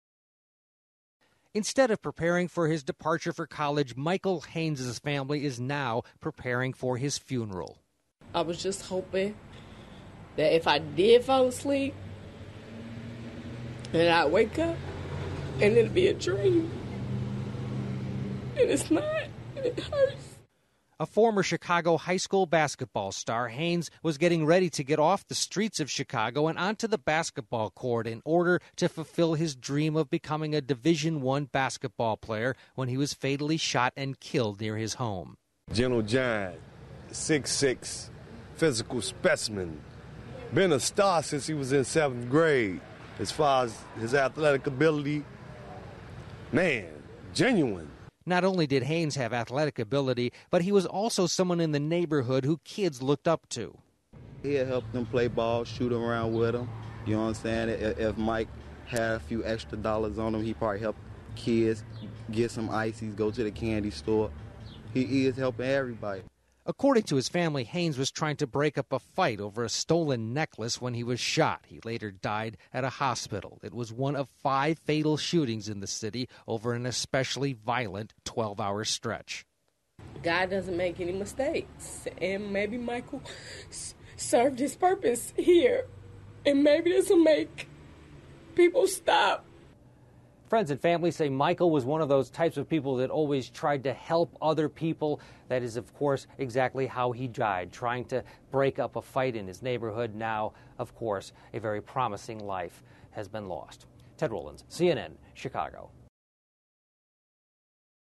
Chicago high school basketball star killed
A former high school basketball star has become another statistic of the violent crime that is taking over Chicago.